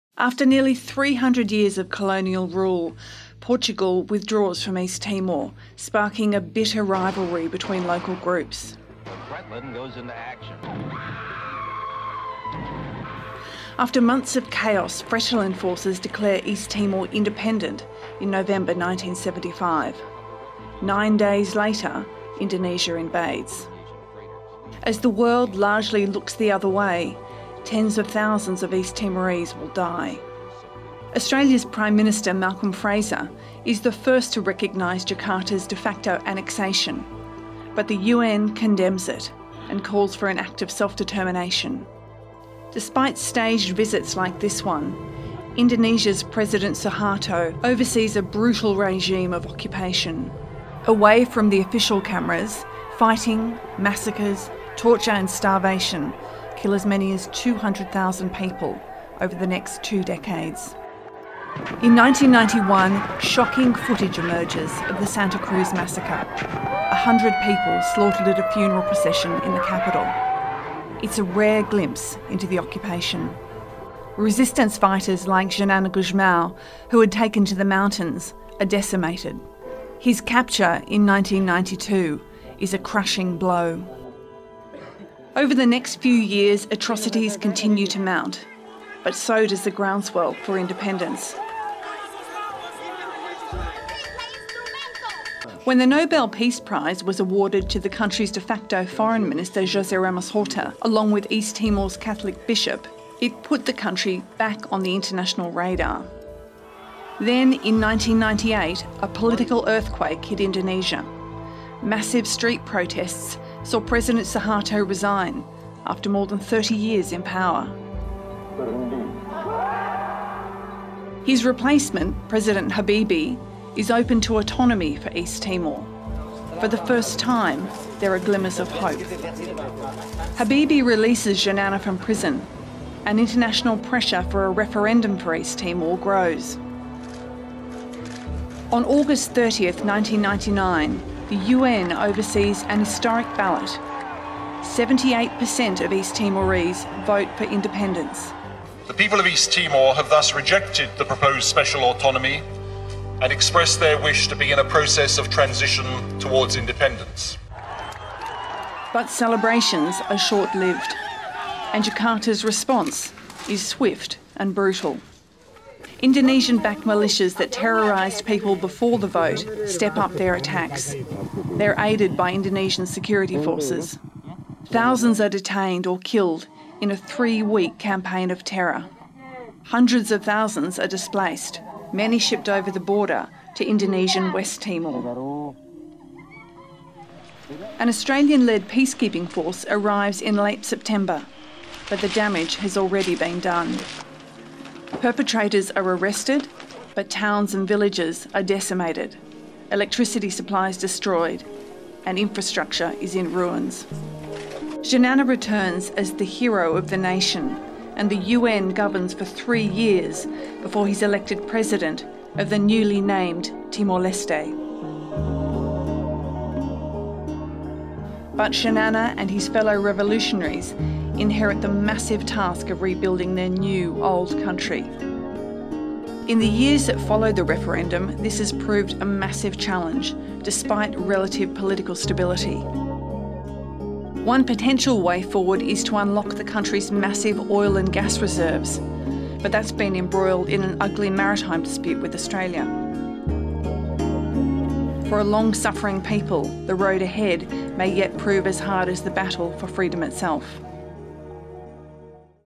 East Timor independence: a short history of a long and brutal struggle
After nearly 300 years of colonial rule, Portugal withdrew from East Timor in 1975, sparking a bitter rivalry between local groups that ended in Fretilin forces declaring East Timor independent. Indonesia invaded nine days later, beginning an brutal regime of occupation that would last until 1999. More than 200,000 East Timorese died and many more suffered during massacres, torture and starvation. In the 1990s, a combination of the independence movement gaining strength, growing worldwide pressure and political reform in Indonesia led to a UN-sponsored referendum on independence on 30 August 1999.  The historic ballot saw 78% of East Timorese vote for independence. Indonesian-backed militias responded by unleashing a wave of terror with attacks on civilians, looting and burning of homes until an Australian-led peacekeeping force arrived in late September 1999.
• East Timor: Indonesia's invasion and the long road to independence • Paul Daley: Australia cast itself as the hero of East Timor. But it was US military might that got troops in